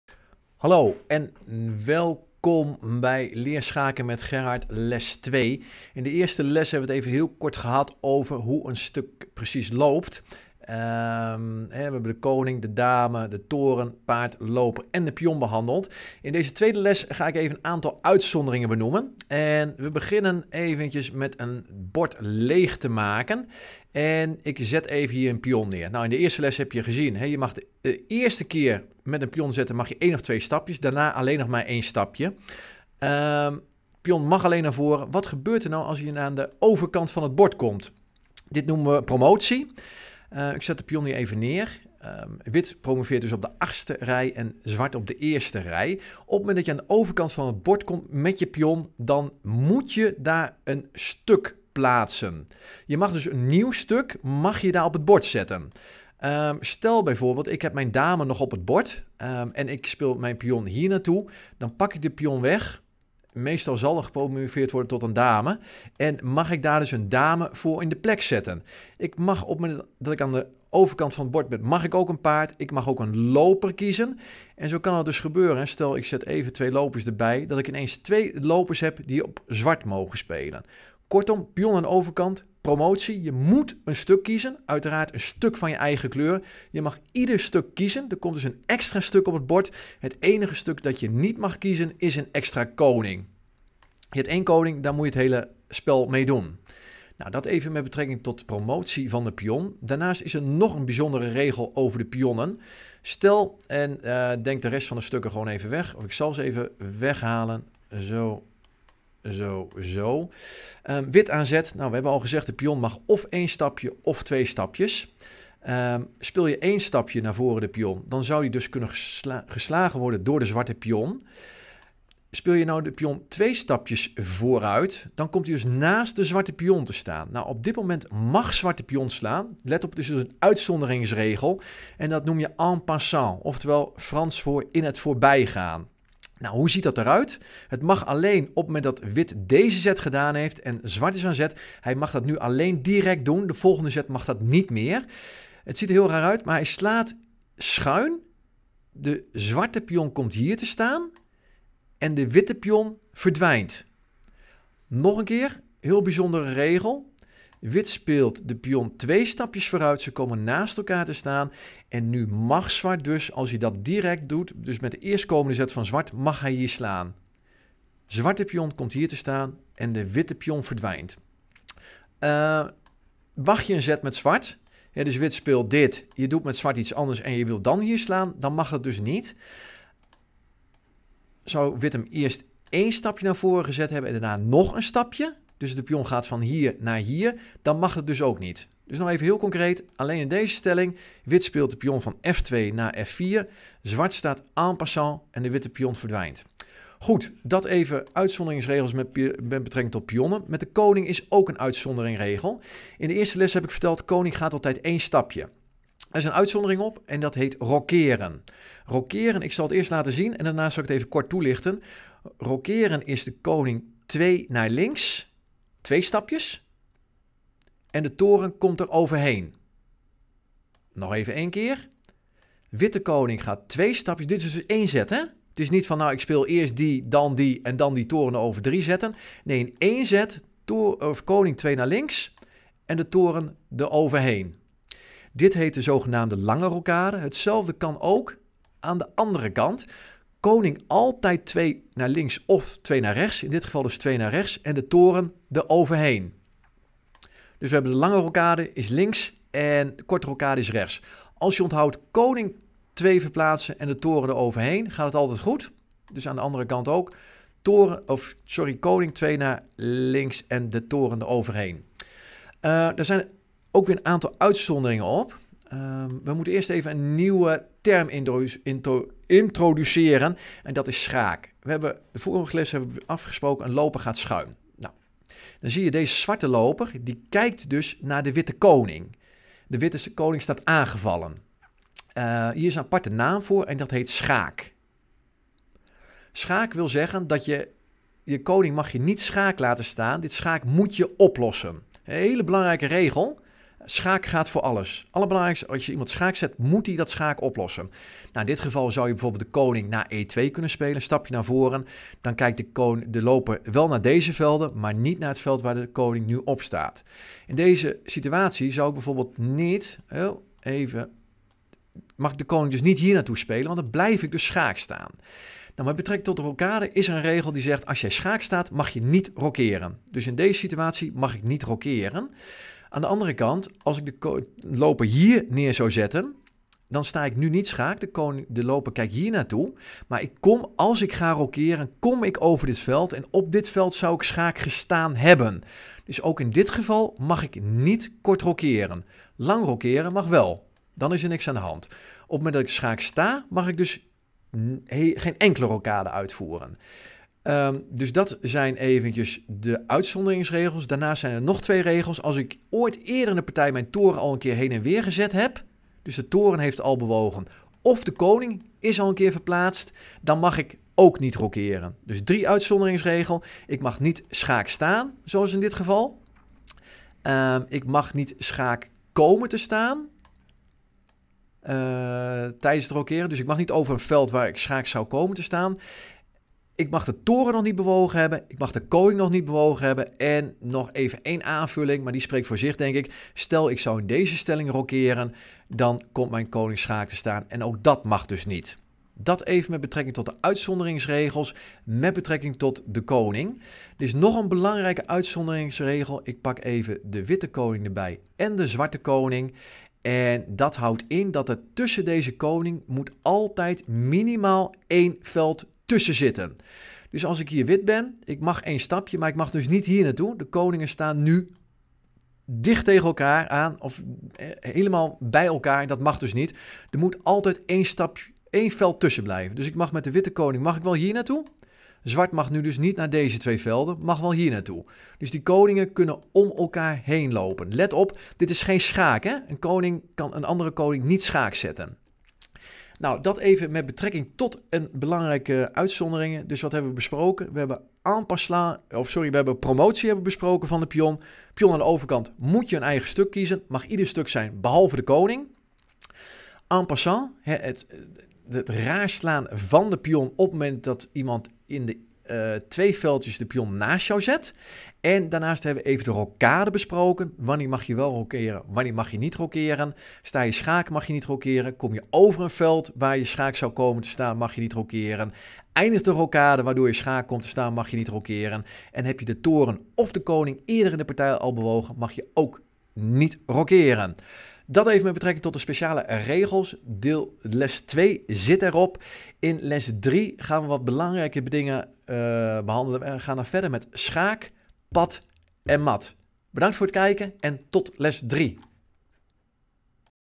schaakles 2 || speciale regels || promoveren || en passant || rocheren
In schaakles 2 leg ik een aantal bijzondere regels en uitzonderingen uit op de regels van de eerste les. Promotie is wanneer je pion aan de overkant van het bord komt. En passant is een uitzonderingsregel op hoe pionnen normaal bewegen en rocheren is een zeer bijzondere zet, waarbij de koning en toren in een zet verplaatst worden!